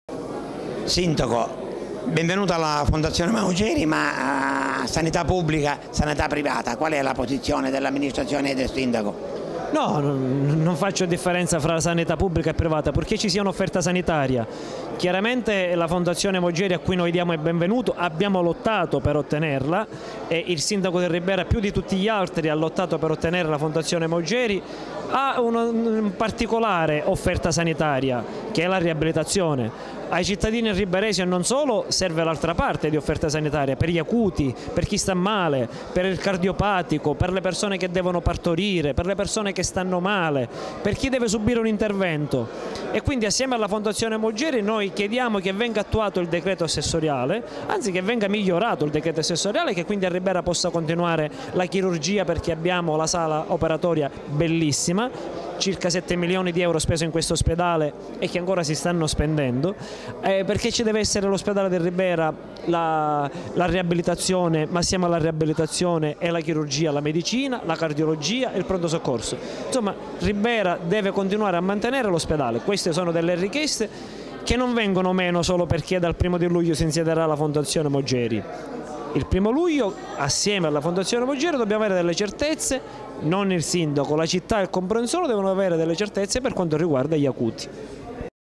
Fondazione Maugeri: parla Carmelo Pace
Video intervista ad Salvatore Messina in occasione della consegna alla Fondazione Maugeri del reparto dell'ospedale che ospiterà un centro d'eccellenza per la riabilitazione.